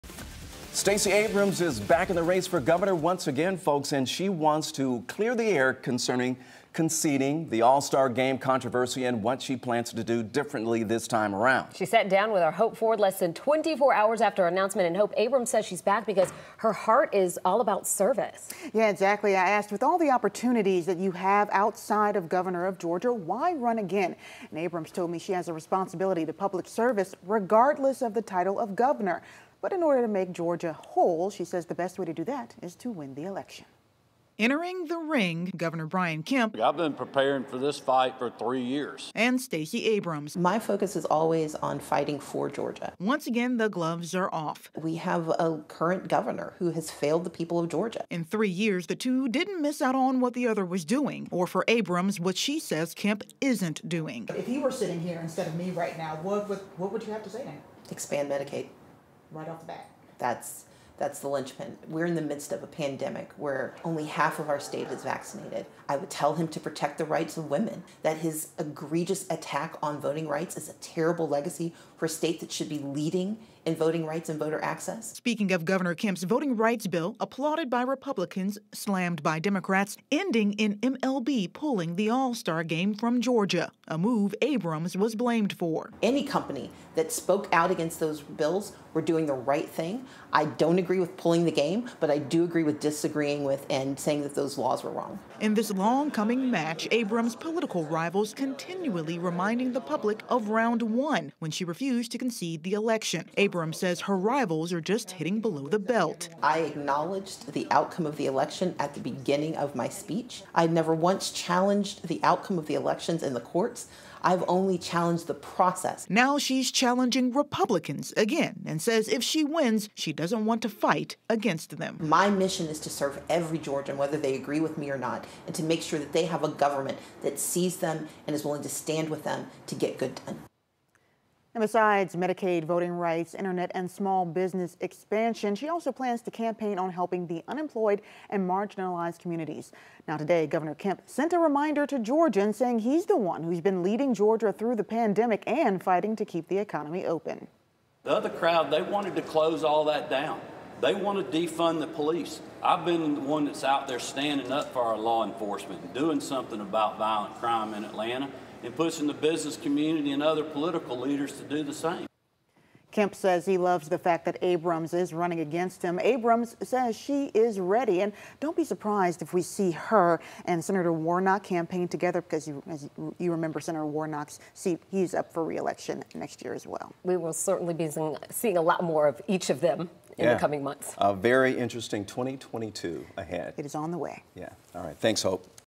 Stacey Abrams running for Governor of Georgia | 1-on-1 interview
Stacey Abrams will look to defeat Brian Kemp as she enters the race for governor of Georgia again.